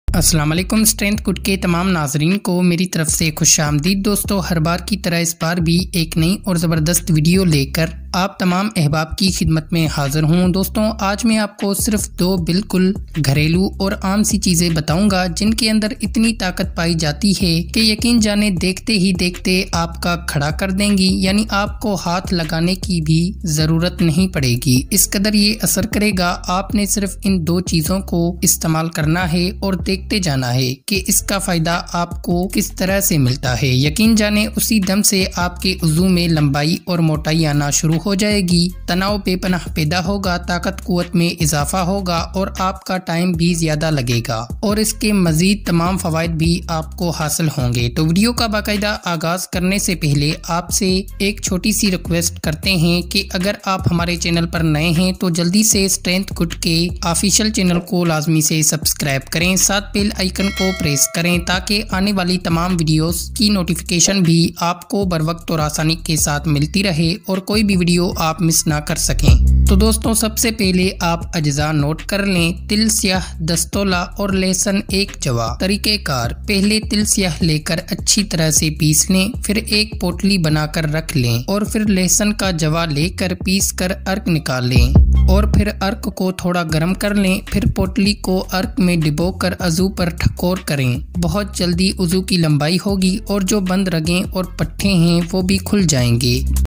Best Health Benefits Of Garlic & Black Sesame Seeds For Weight Loss & Skin || Strength Good
Hello friends,

today we have told you In this Vedio some amazing health benefits of garlic & sesame seeds